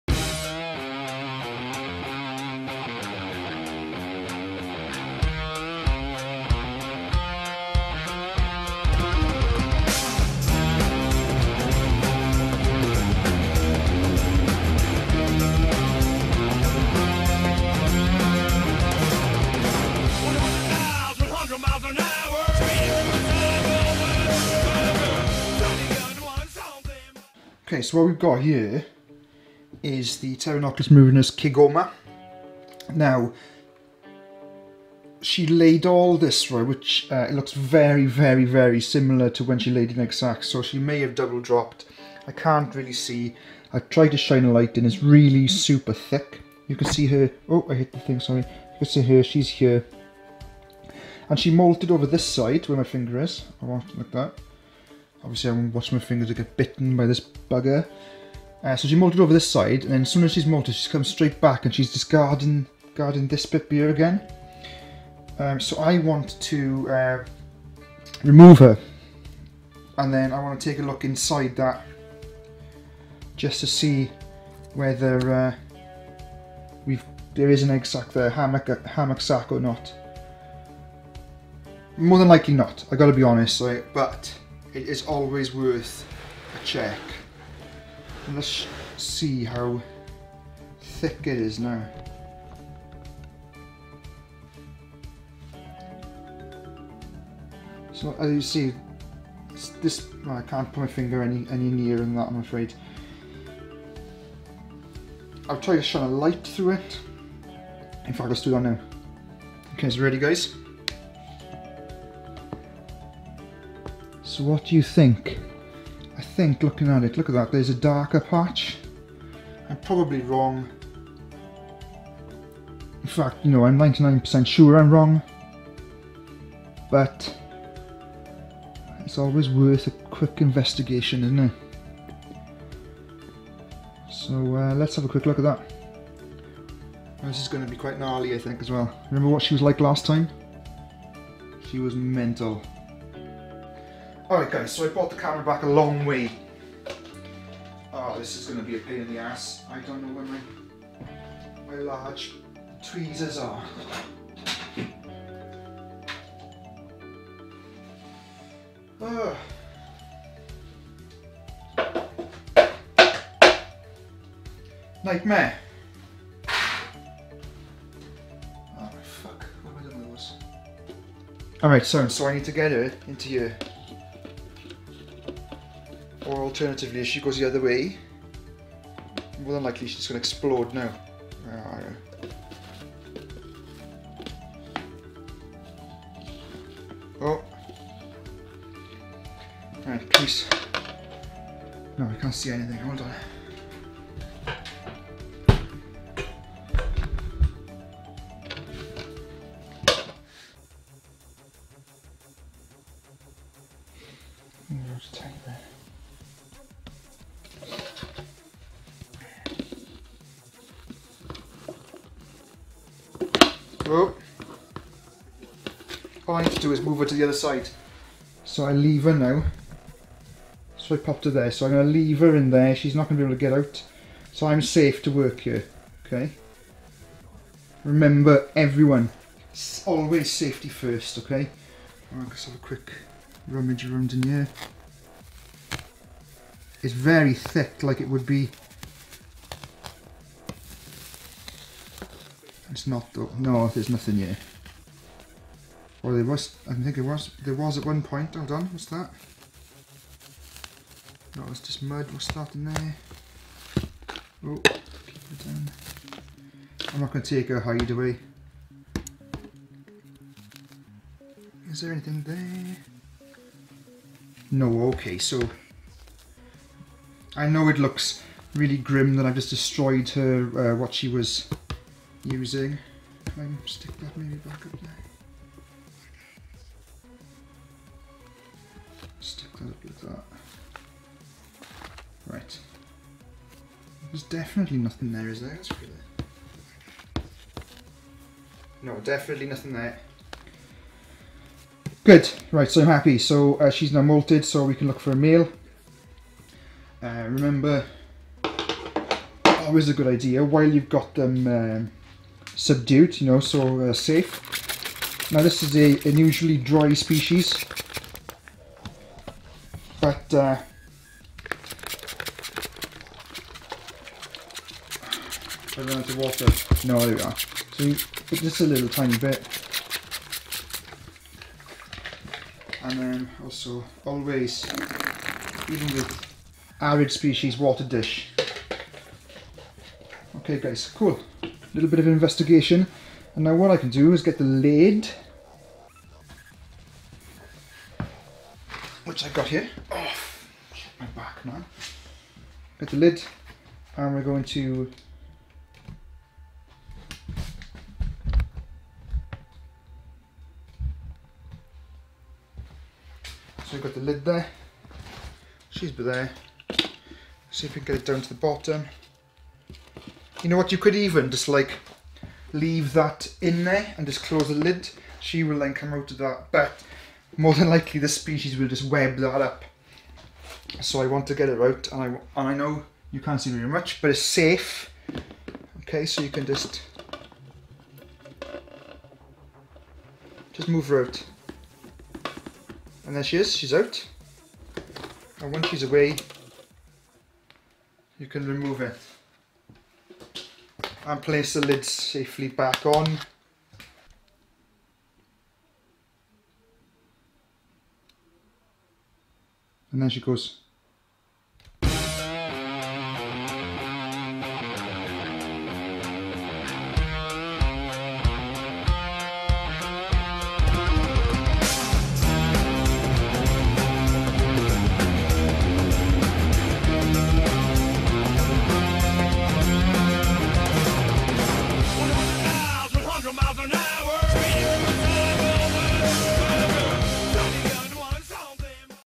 Looking for a Pterinochilus EGGSAC - Moving the ORANGE BITEY THING safely
The colour of this species varies depending on original locality. They vary from a beautiful bright orange colour to tan with a starburst pattern covering its body. It is very interesting in both its behaviour and habits. Not a beginners species but definitely one of those species you need to keep sooner or later. They do not require high humidity and can be kept on a slightly moistened substrate with a water dish. They are best kept in a terrarium that is set up with both arboreal and terrestrial hides. If given enough substrate they may even dig a burrow. They are very heavy webber's and will not only cover a dense layer of web over their retreat but also over the entire enclosure, establishing elaborate silken tunnels. Care needs to be given when moving them and maintaining the enclosure as they are very fast and will not hesitate to bite or escape.

Amazon Wishlist
BUY some Merch - shop.spreadshirt.co.uk/arachnotube